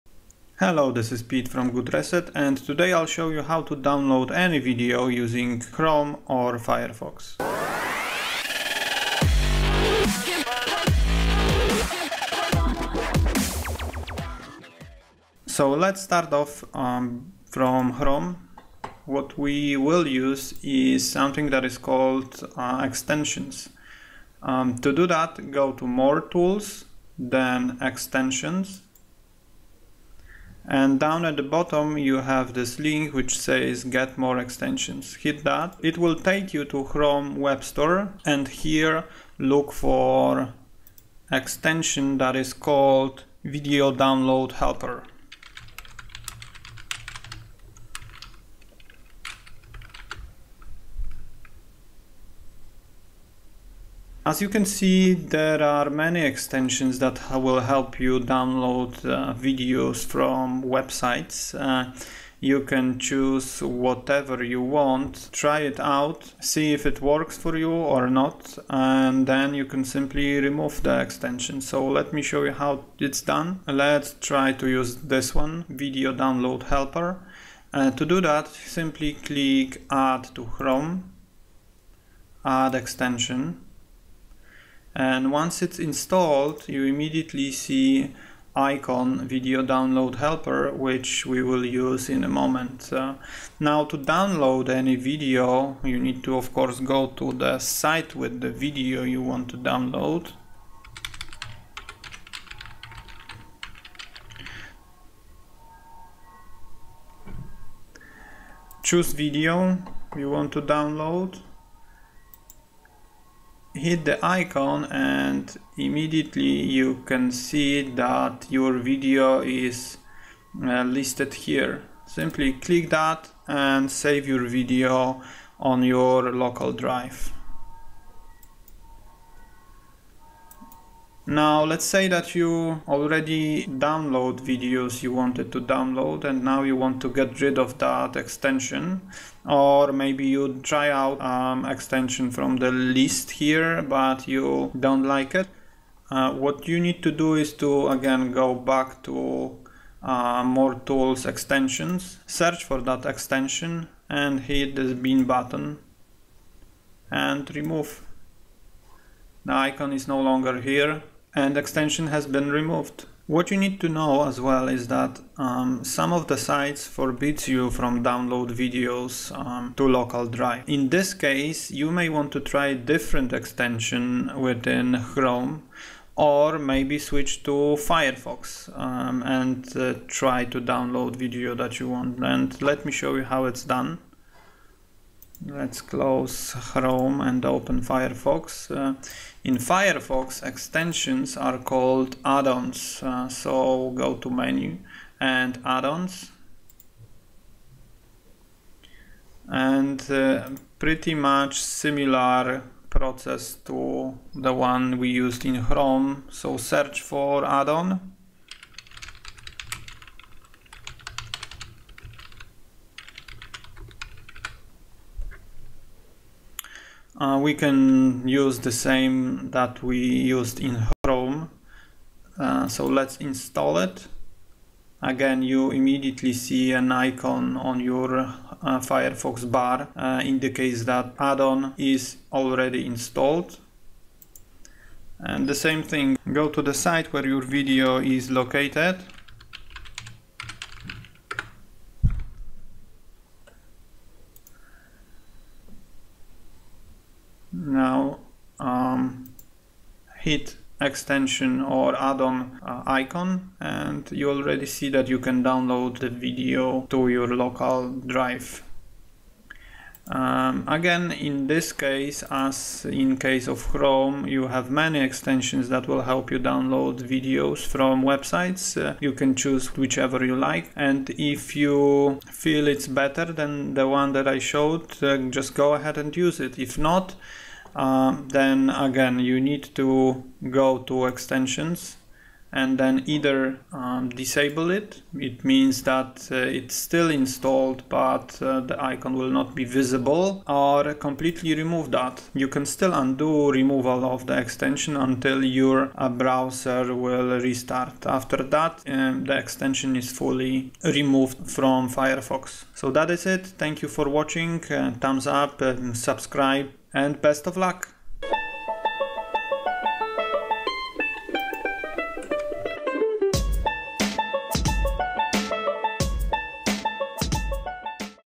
How to Download Video from any site using Chrome and Firefox
In this video you can learn how to download video using Chrome or Firefox browsers.
1. Chrome
a) Go to menu - More tools - Extension
b) at the bottom of the Extension site hit Get more Extensions.
c) Make sure that Extensions is selected from left menu
d) in search bar write "video downloader helper and search
e) when you find extension you are looking for hit Add to chrome next to its name.
DONE - extension added
You can now go to the site with video you want to download and use  extension to download that video.

2. Firefox
a) hit menu button top right
b) go to Add-ons
c) make sure that Get Add-ons is selected from left menu
e) when you find extension you are looking for hit Install button